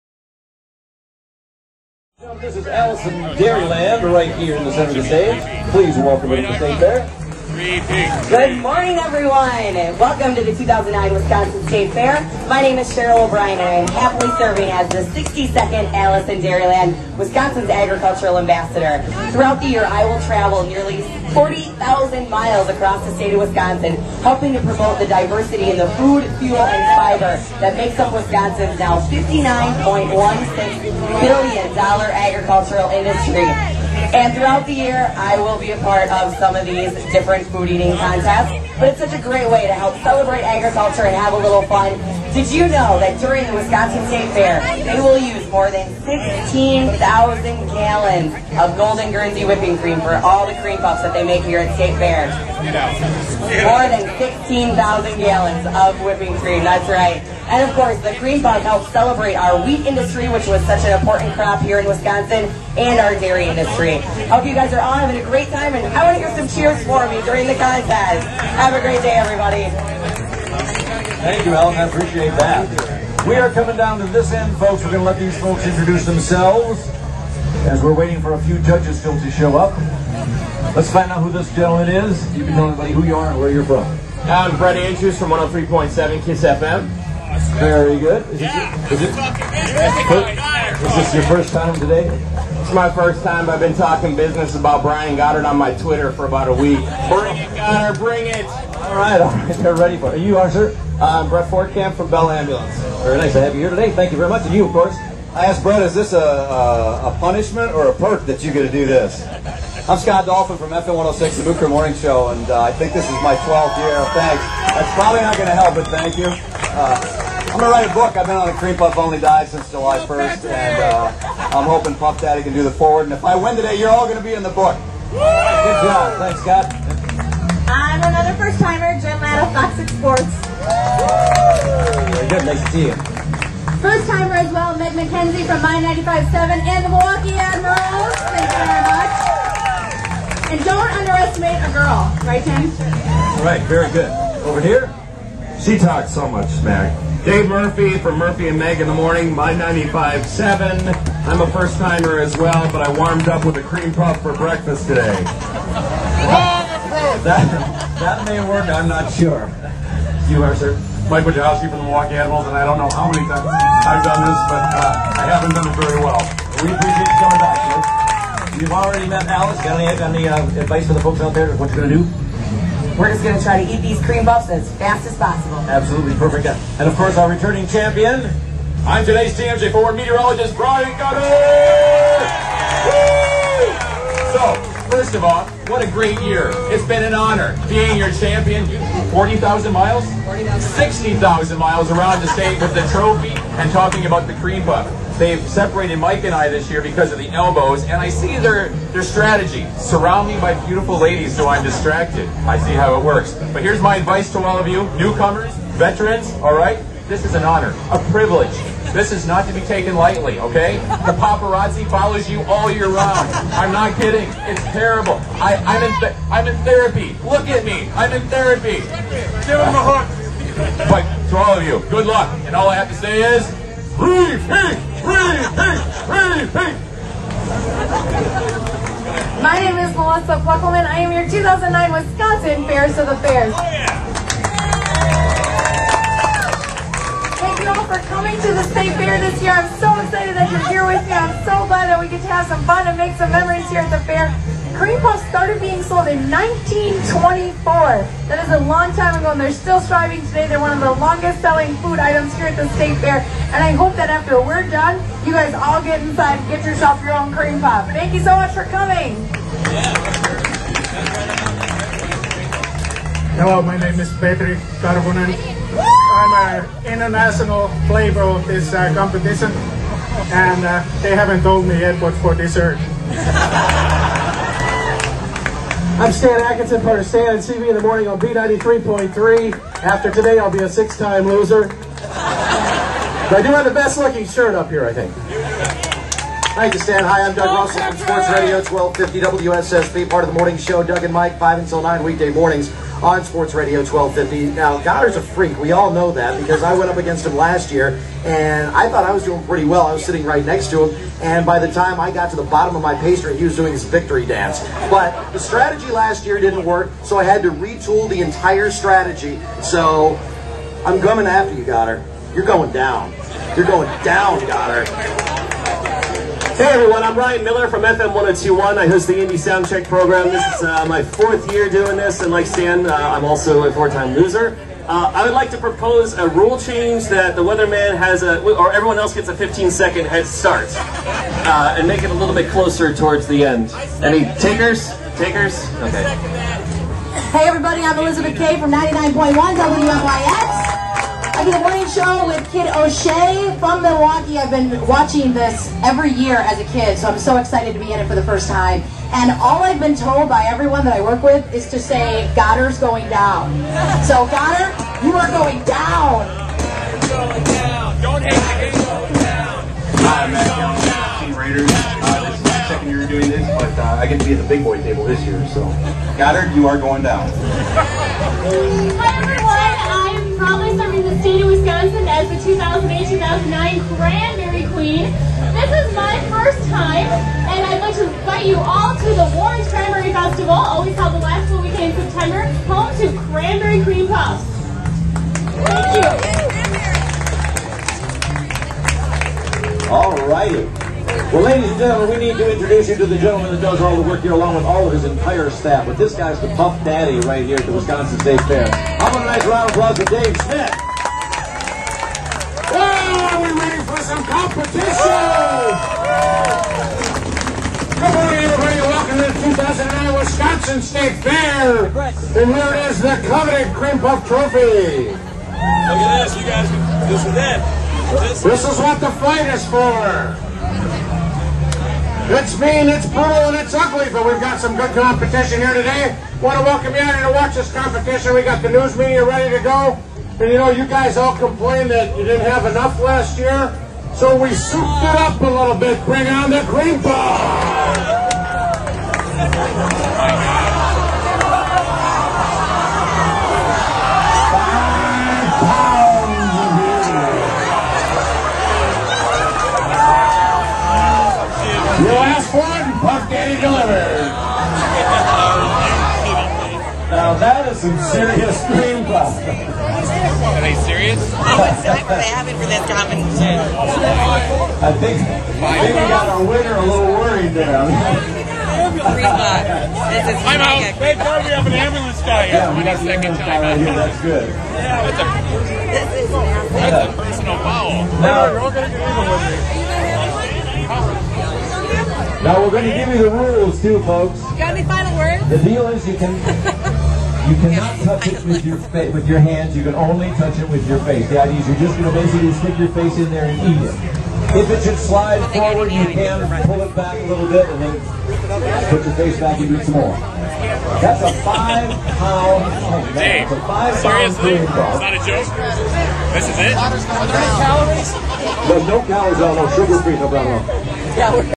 2009 WI State Fair Celebrity Cream Puff Eating Contest Part 1
62nd Alice in Dairyland Cheryl O'Brien was one of many contestants in this year's Wisconsin State Fair Celebrity Cream Puff Eating Contest.  In Part 1, meet the contestants and see what BIG surprise was in store for the contestants this year...